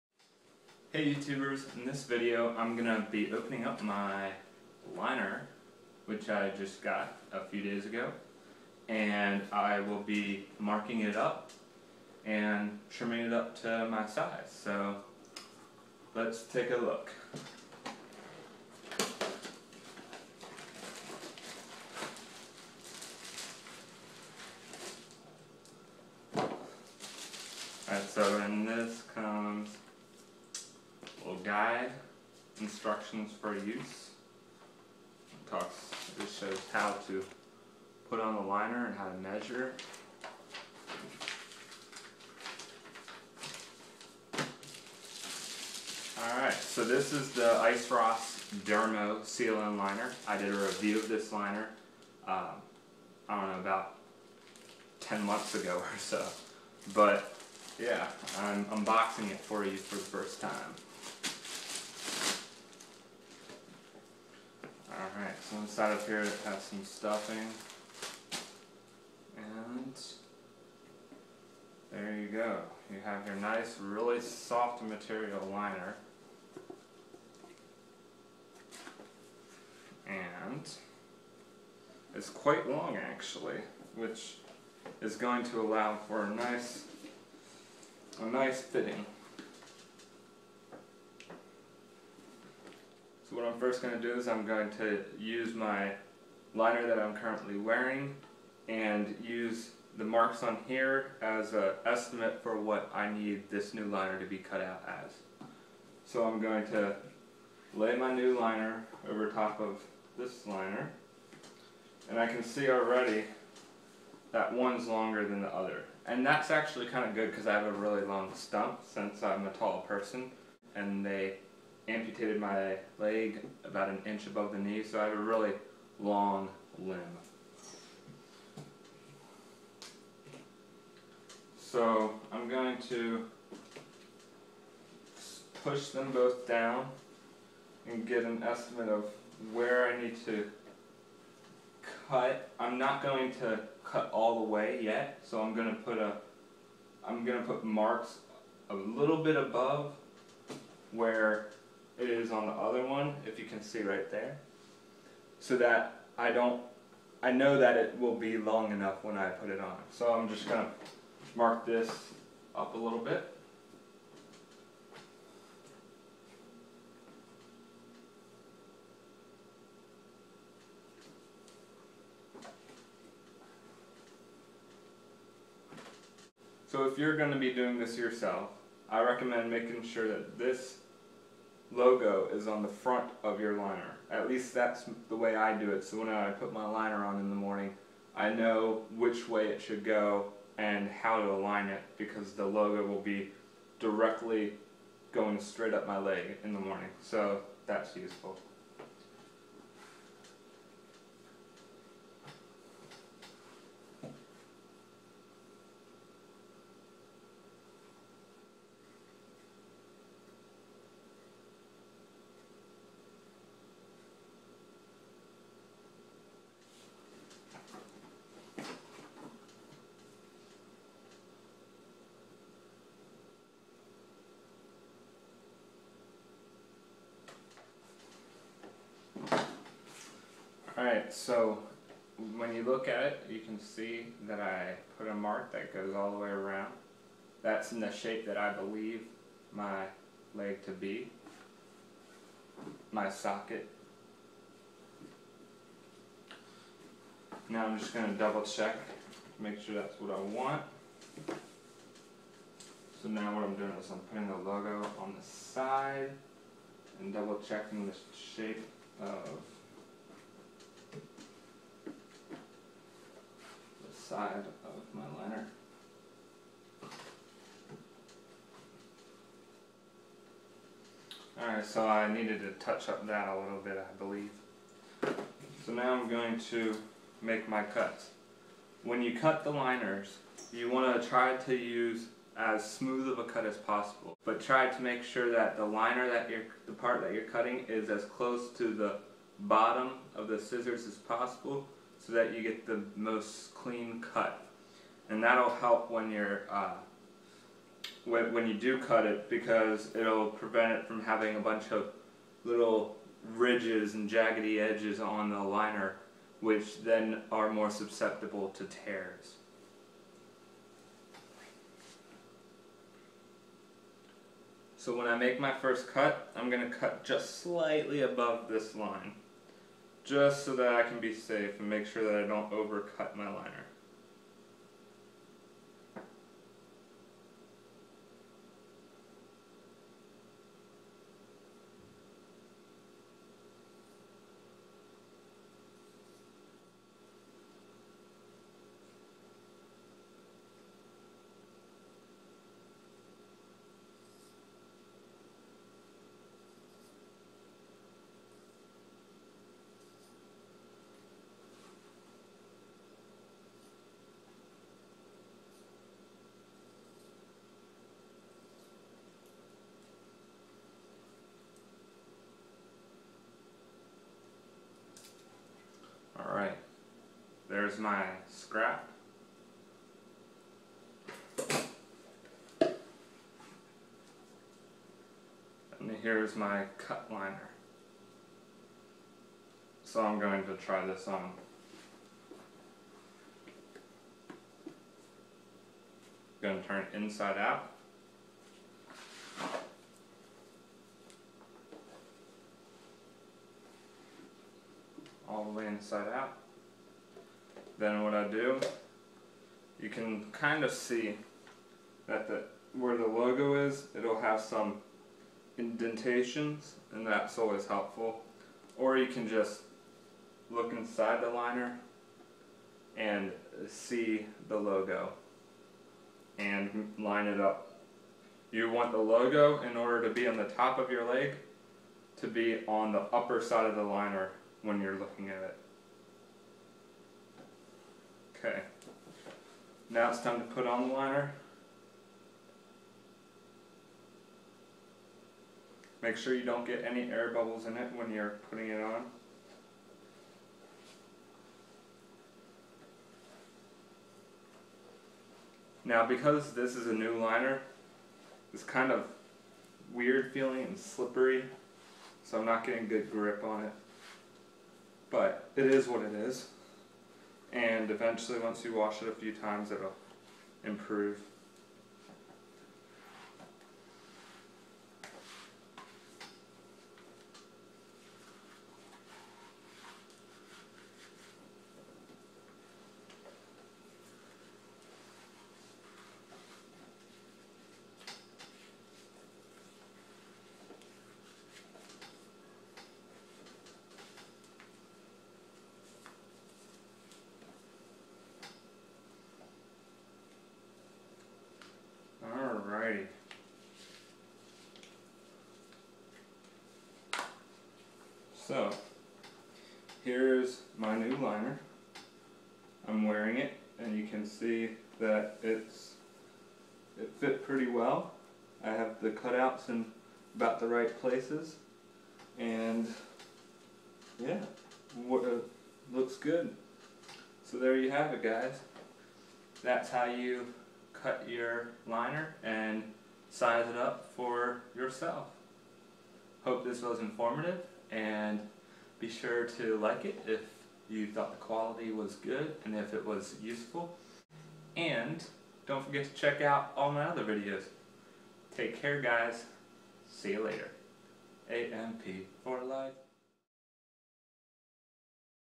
How to Custom Fit a Prosthetic Liner
This is a how to video explaining the process of cutting and custom fitting a prosthetic liner.